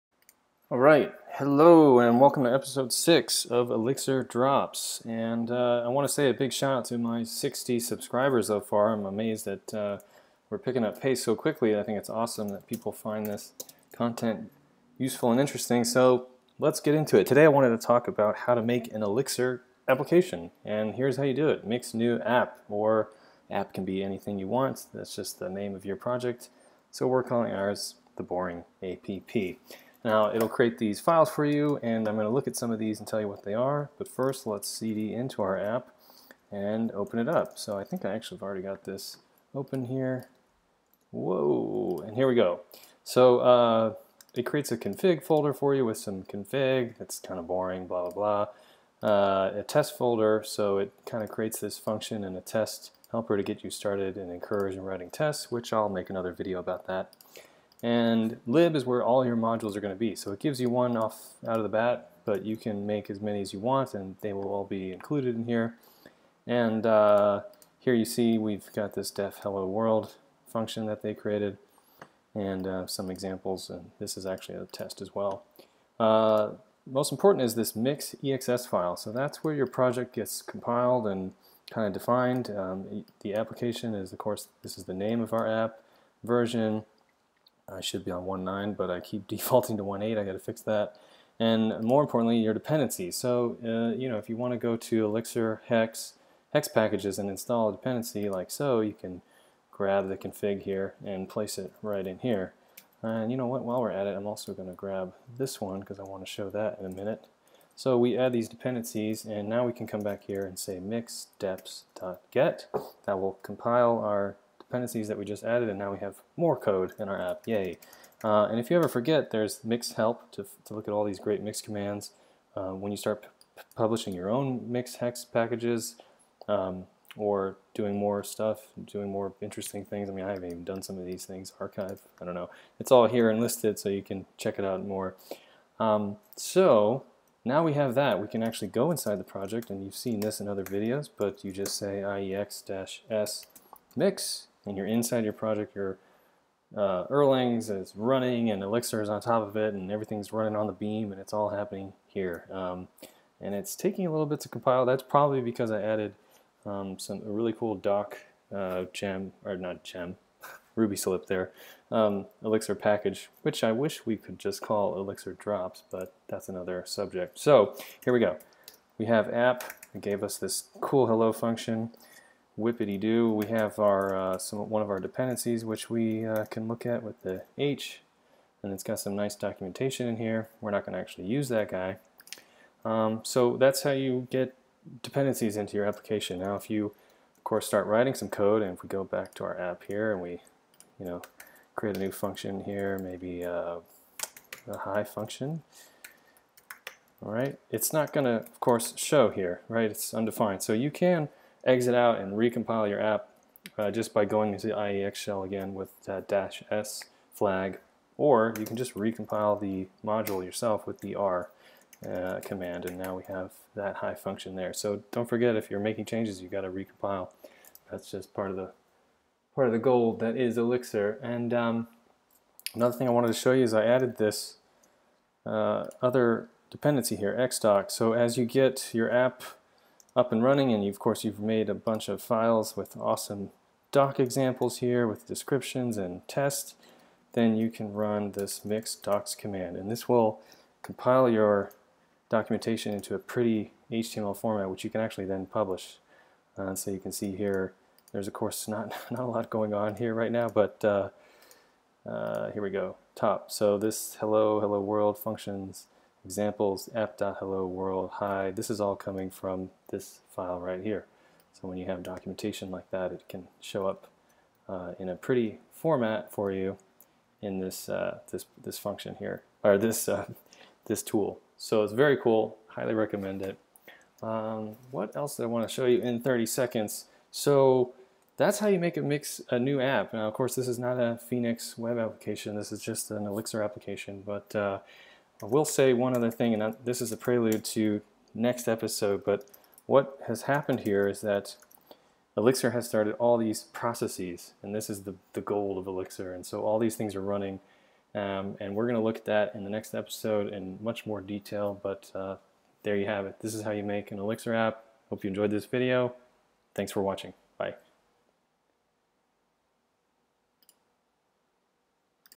Make a New Application in Elixir!
Episode 6 -
We will create a new app using `mix new` and much more!
- iex -S mix
- mix.exs
- deps
- docs